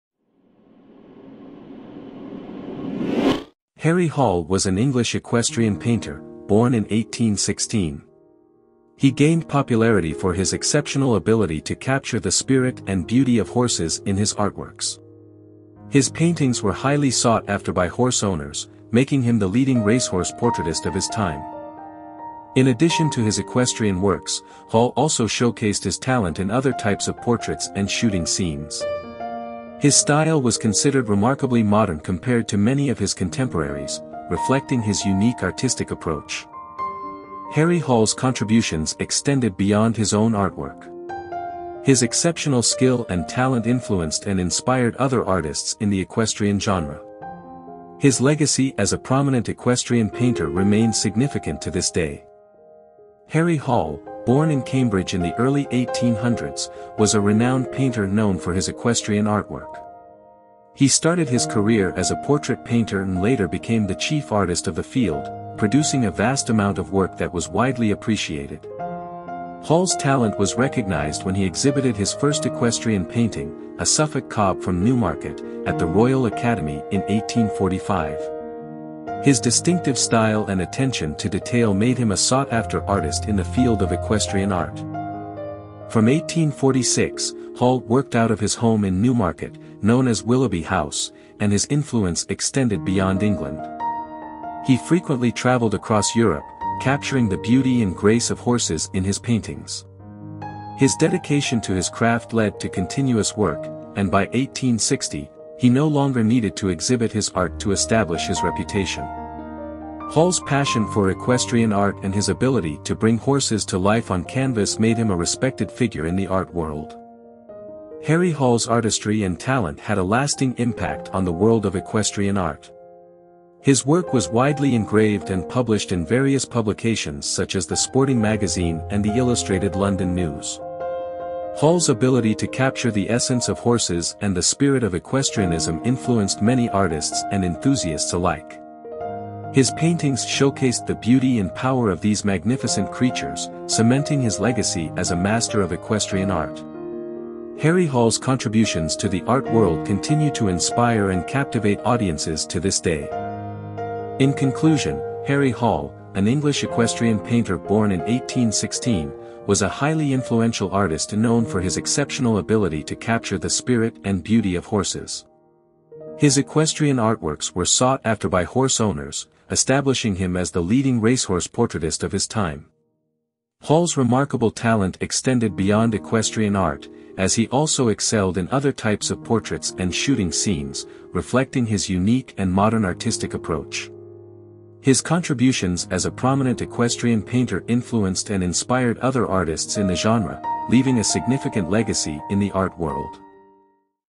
Harry Hall: Capturing the Spirit of Horses｜Artist Biography
Harry Hall was an English equestrian painter, born in 1816

A biography of Harry Hall.